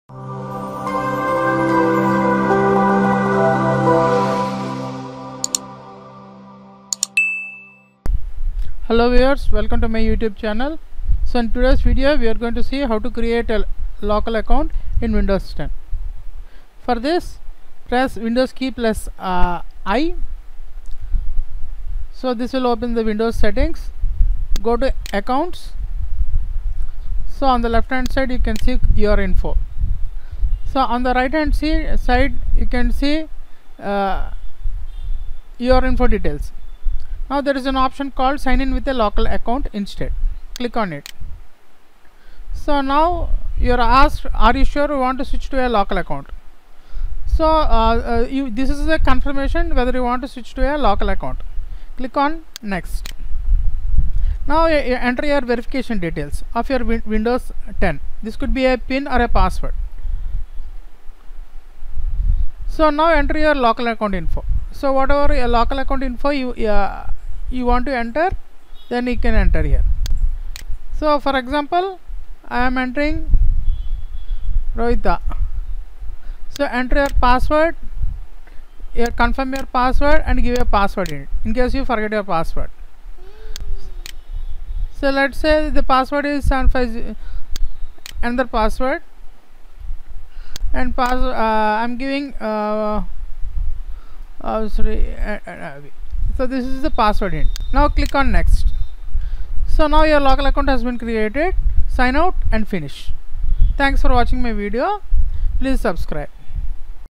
Create a Local Account in Windows 10 - 4 Steps
After the 2004 update, you can still login to your Windows 10 computer using a local account.  This is not connected to the Internet and will speed up your computer usage.

Although your require Microsoft account for different services like Microsoft Edge, OneDrive, Office and other apps, local account keeps all your files offline.

Also, for privacy and security concerns, Windows 10 users may prefer a local account. This will also prevent collection of your data for ads or other means.

Press Win + I keys to open Windows Settings.
Open Accounts - Your Info
Sign in with a local account instead is visible, on the right-hand side.